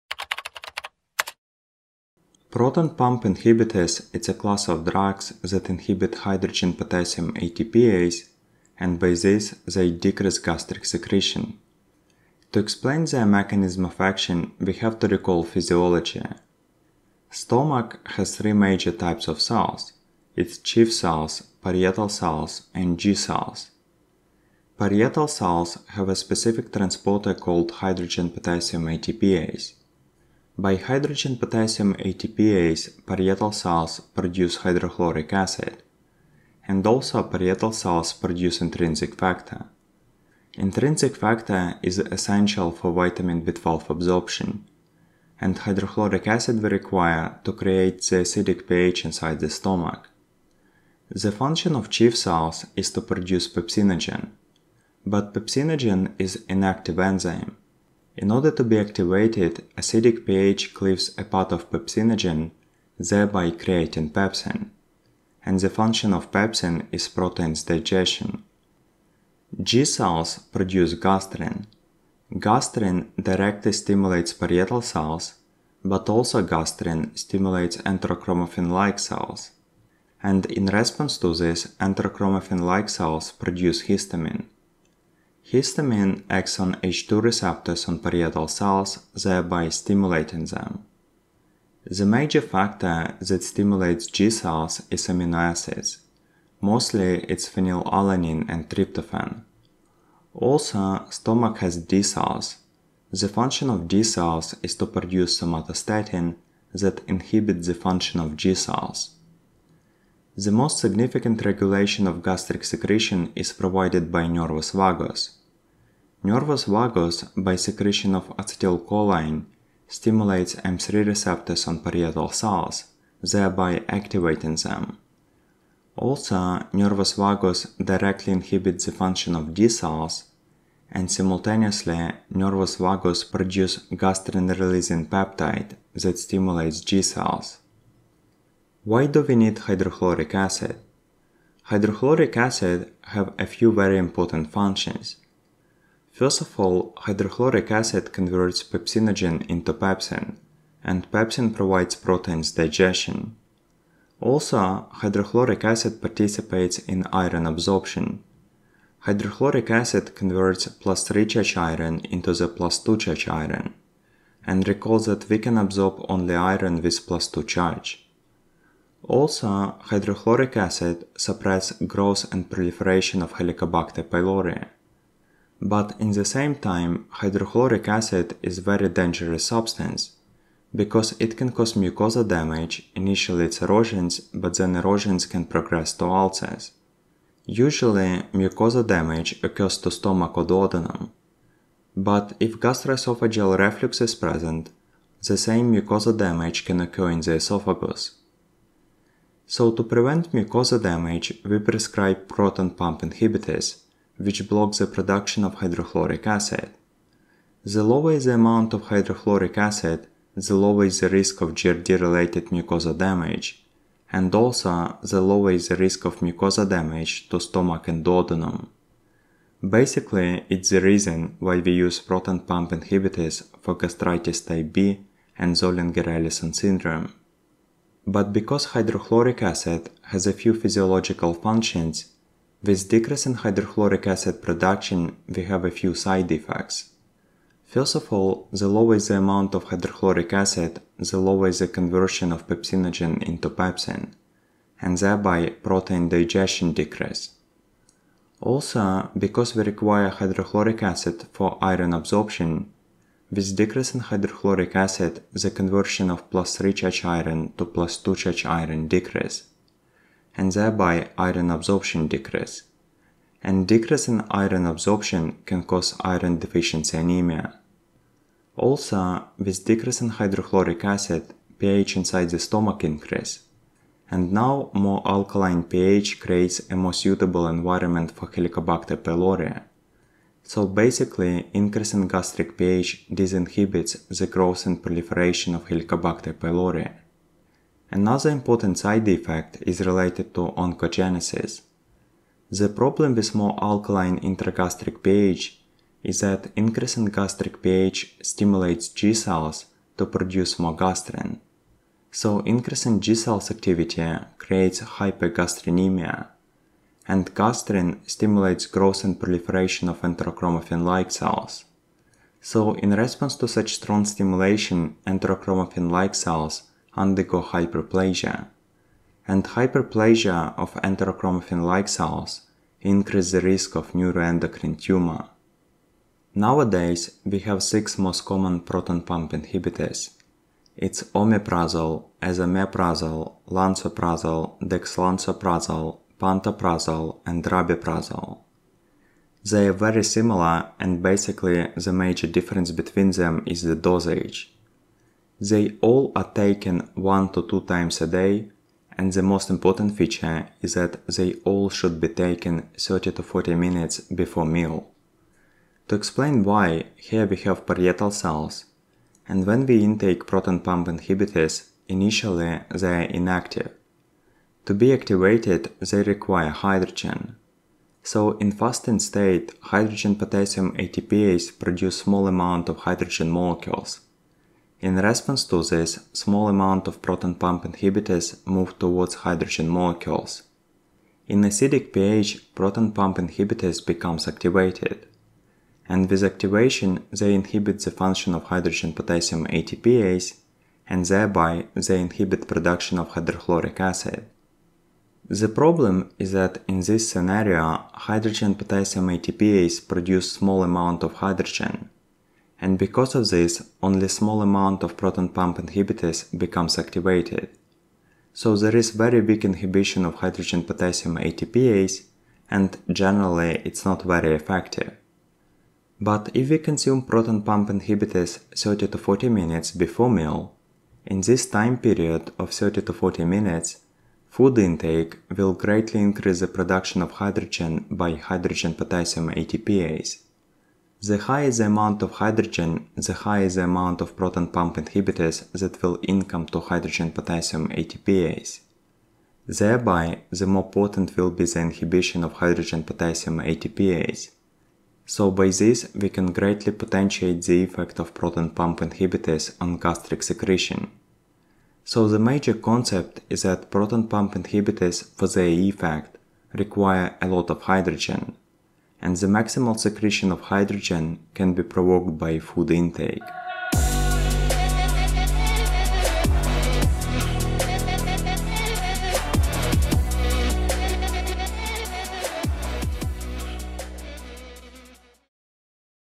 PROTON PUMP INHIBITORS Mechanism of action. Side effects Explained
0:00 Physiology of Gastric secretion
2:17 Hydrochloric acid functions
3:18 PPI’s mechanism of action and side effects
4:52 Hypergastrinemia and Neuroendocrine tumor (NET) risk
5:48 Why we take PPI’s 30-40 minutes before meal